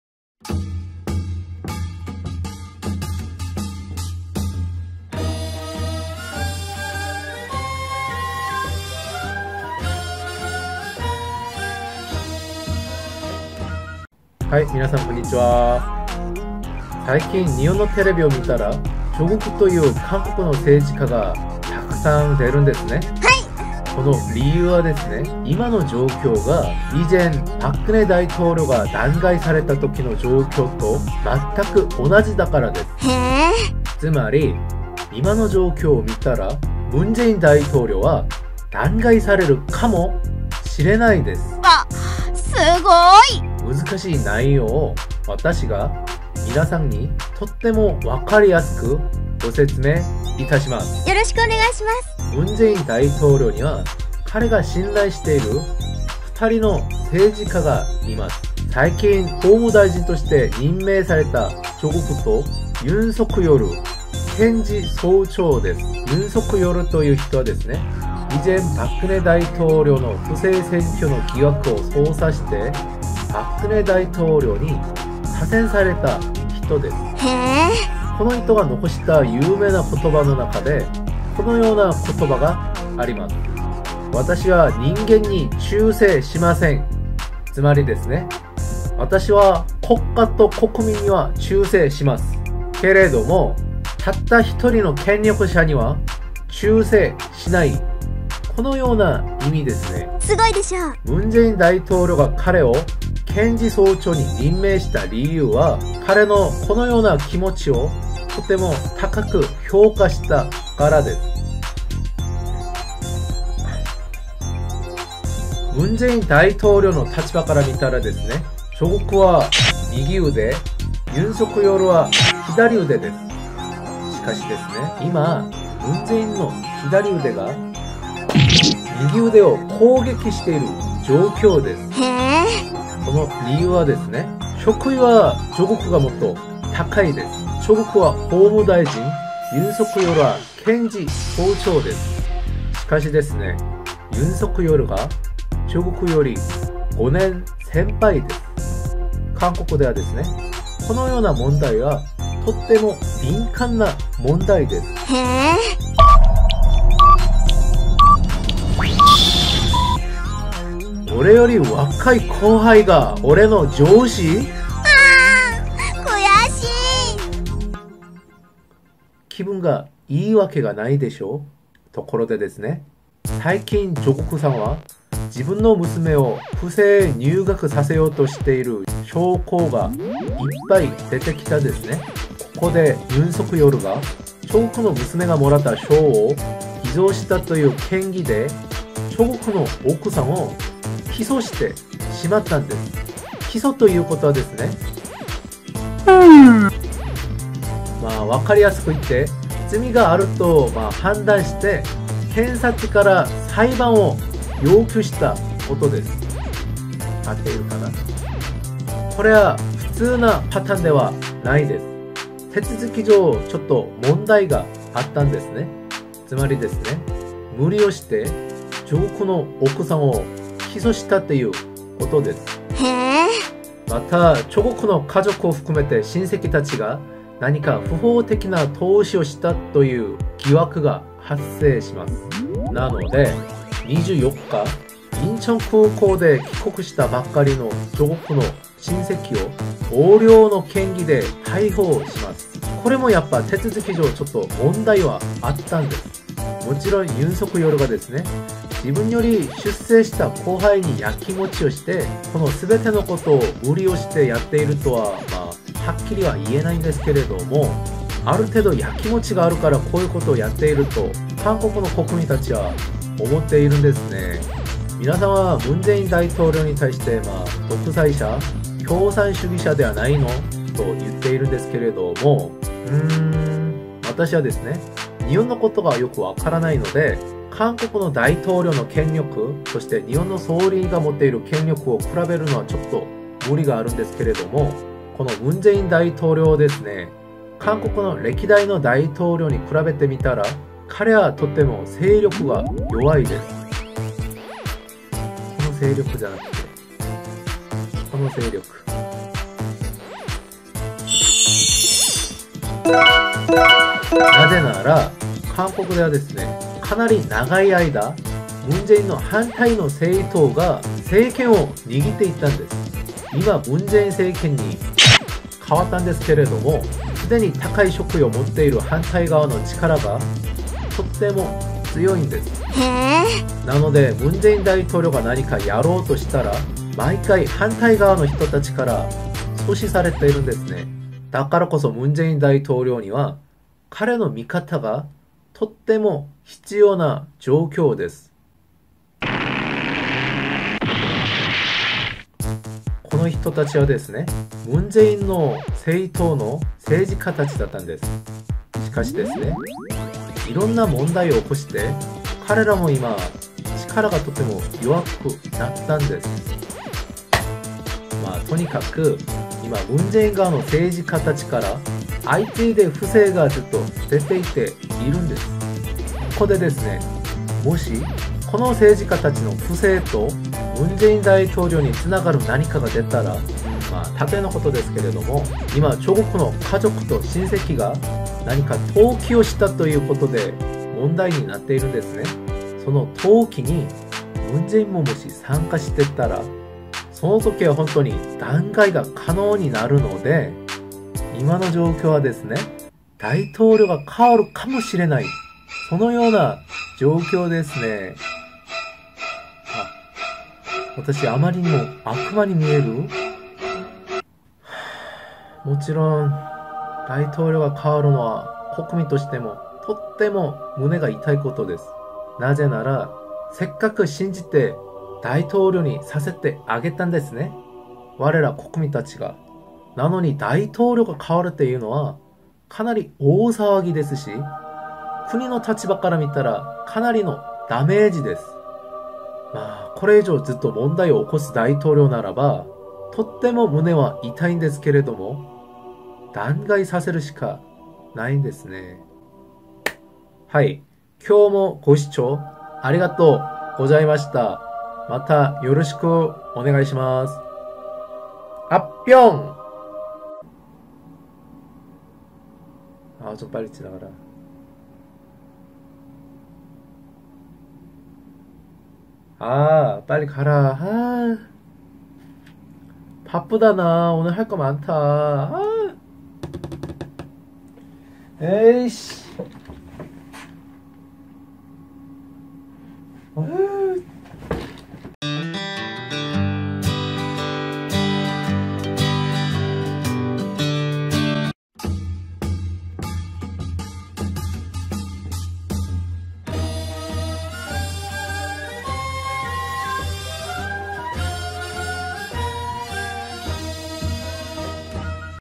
チョグクの親戚逮捕で韓国大統領の悩みが増えた
韓国男子TV

韓国の政治家チョグクの親戚が逮捕された
これで文在寅の弾劾がもう一歩進んだ